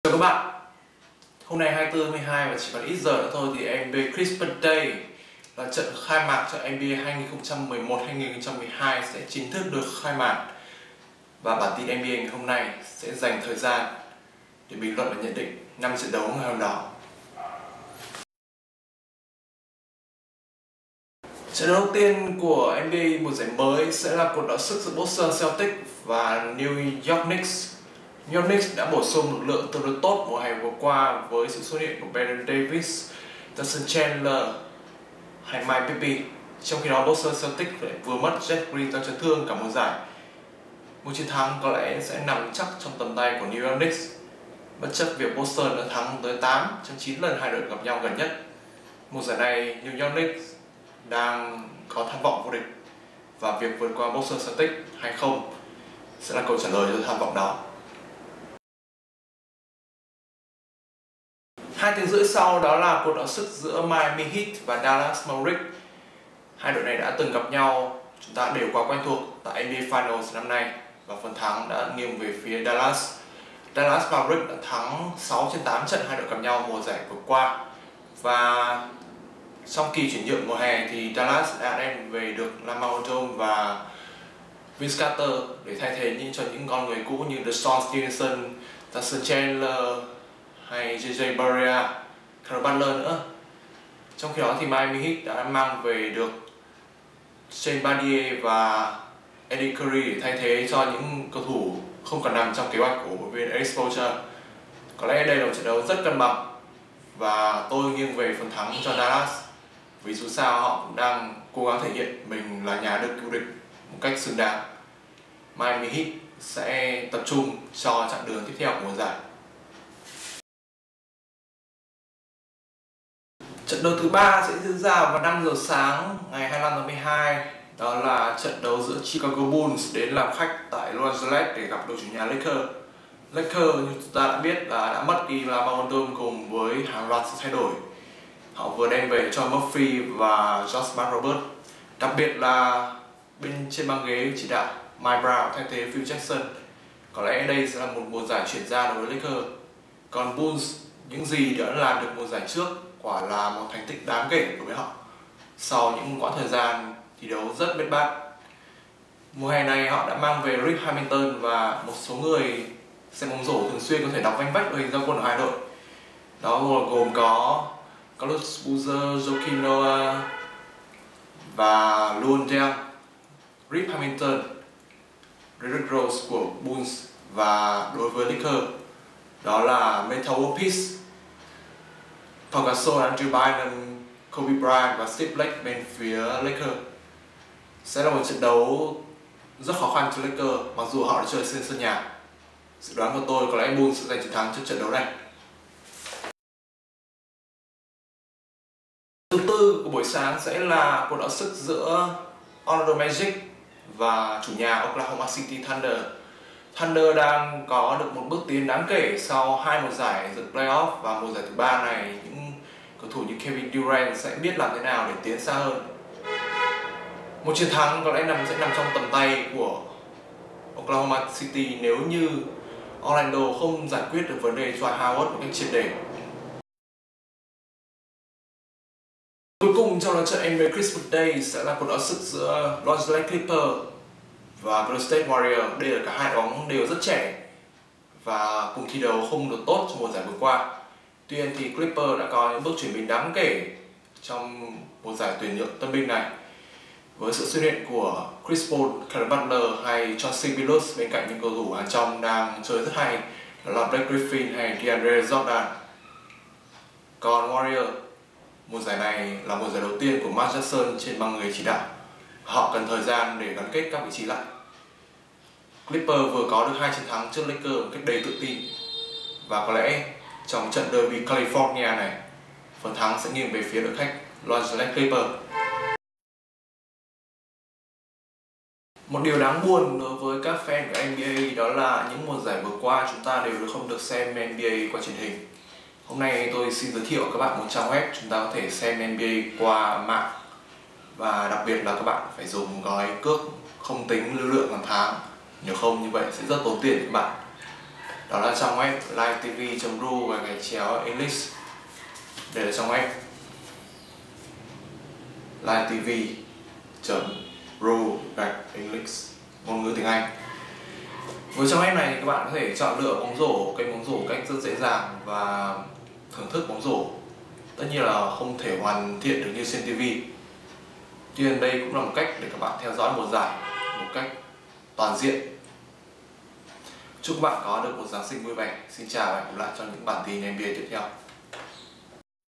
Bản tin NBA 24/12/2011: Christmas Day Preview.
Nhận định trước 5 trận đấu ngày Giáng Sinh- mở màn NBA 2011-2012